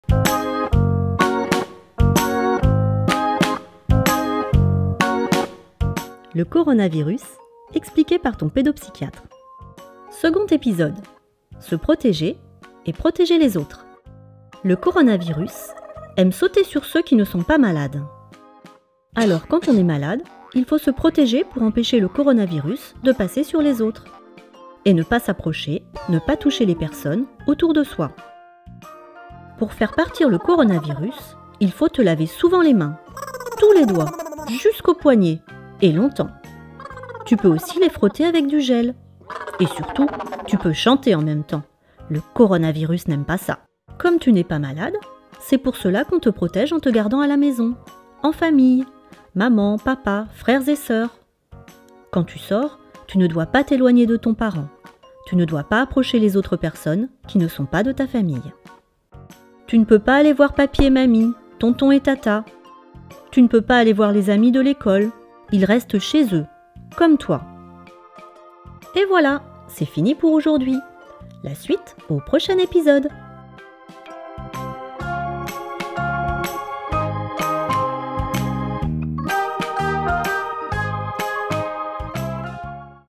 Parler du coronavirus aux enfants : se protéger et protéger les autres !
Second épisode d'une mini-série de clips destinés aux enfants, cette vidéo a été adaptée de l'excellente bande-dessinée, "Le coronavirus expliqué par ton pédopsychiatre" publiée par la Société d'Information Psychiatrique et illustrée par Florian Proust.